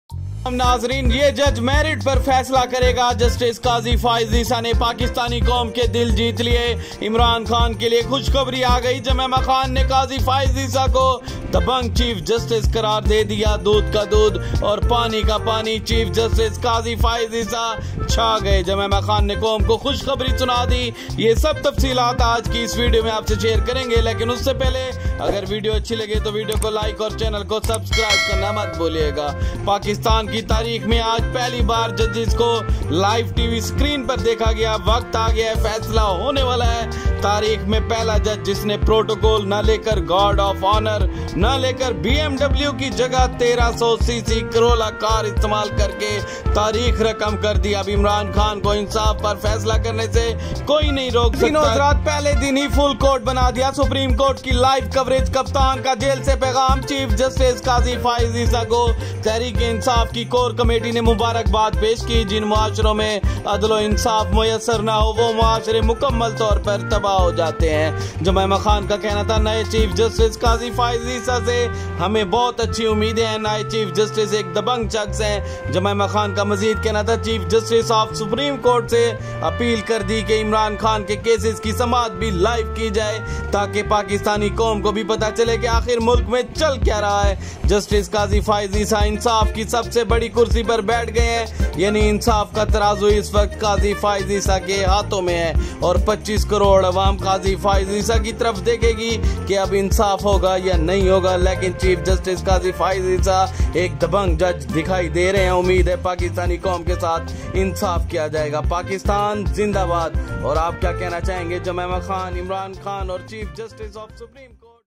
Cheif Justice Qazi Faez Isa Well Wishes From Jemima Khan
Cheif Justice Of Supreme Court Qazi Faez Isa Well Wishes From Jemima Khan